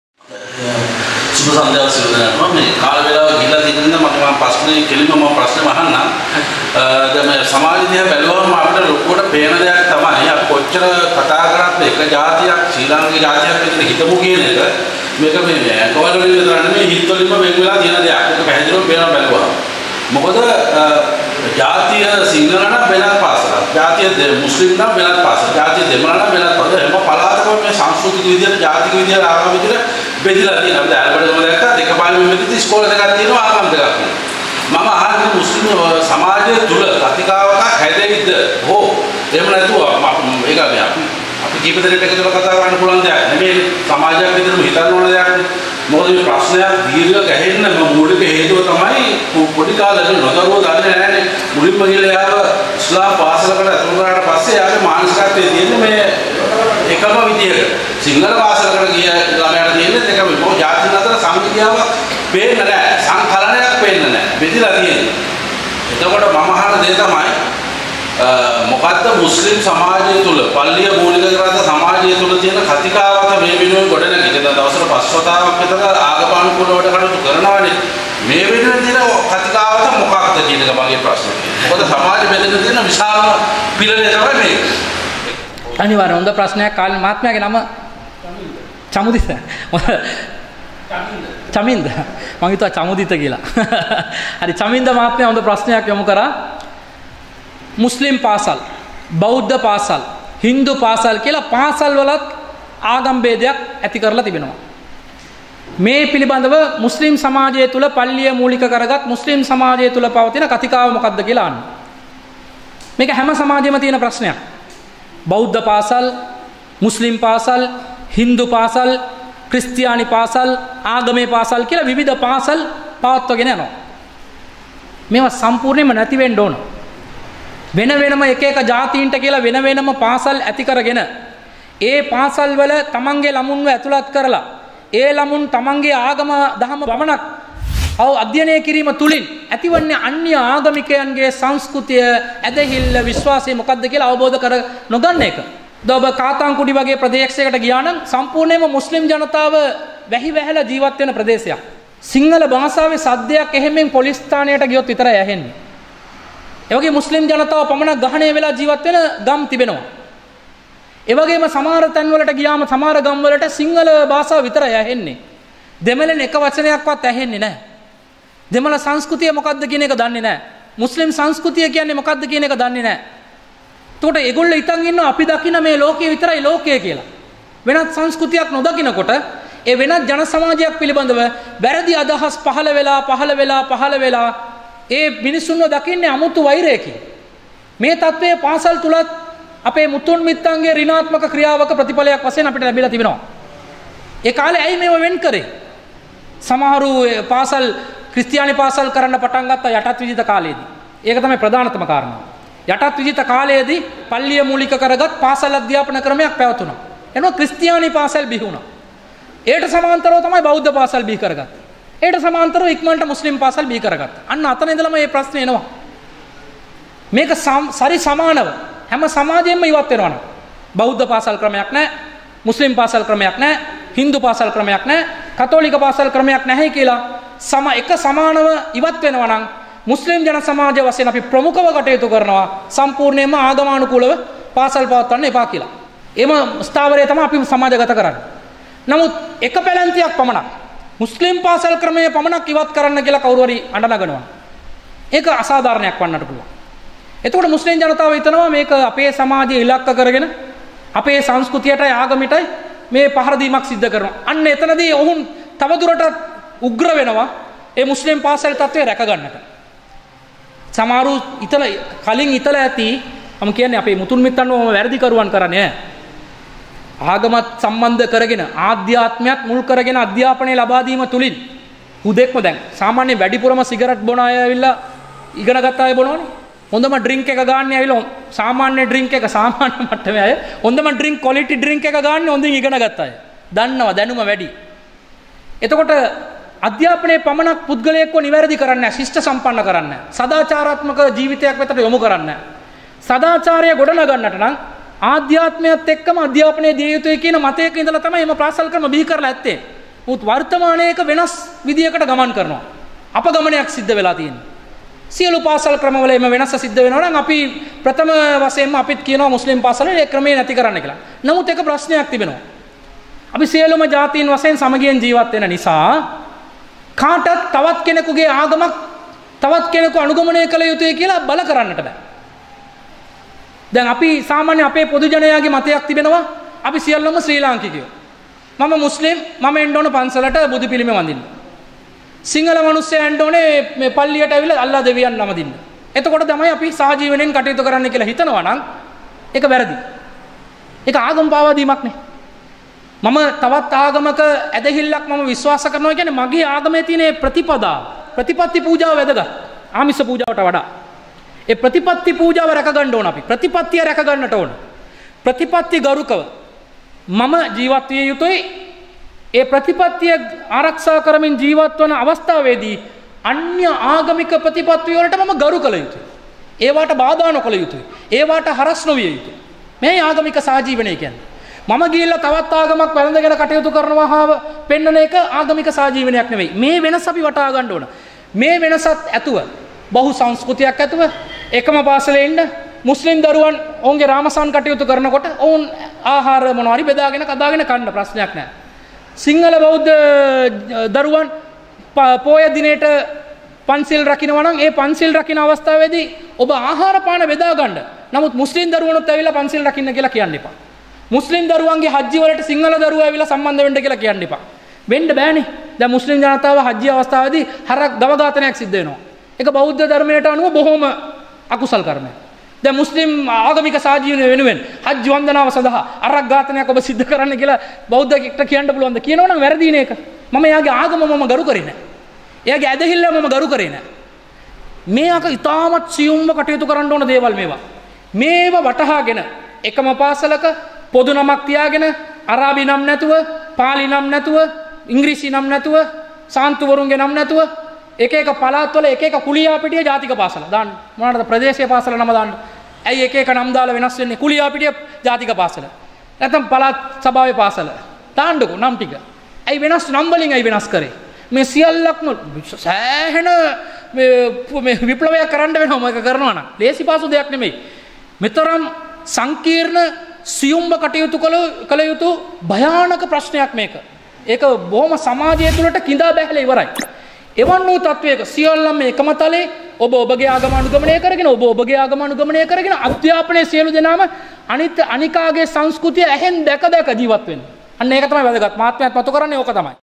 ජාති, ආගම් අනුව පාසල් බිහි වීම පිළිබඳ පල්ලිය මුල් කර ගත් මුස්ලිම් සමාජය තුළ පවතින කතාකාව කුමක් ද? ᴴᴰ
ජාති, ආගම් අනුව පාසල් බිහි වීම පිළිබඳ පල්ලිය මුල් කර ගත් මුස්ලිම් සමාජය තුළ පවතින කතාකාව කුමක් ද? ᴴᴰ - සහෝදර චමින්ද, ඇහැලියගොඩ.

ඇලබඩගම සාමයික සහජීවන වැඩසටහන!

ආර්. අබ්දුර් රාසික්
වාණිජවේදී (කැලණිය), නීතිවේදී (එක්සත් රාජ්‍යය), මනෝවිද්‍යා හා උපදේශන ඩිප්ලෝමාධාරී, සිංහල ශාස්ත්‍රපති (කොළඹ) කියවීම

Video@YouTube#abdurrazikbayan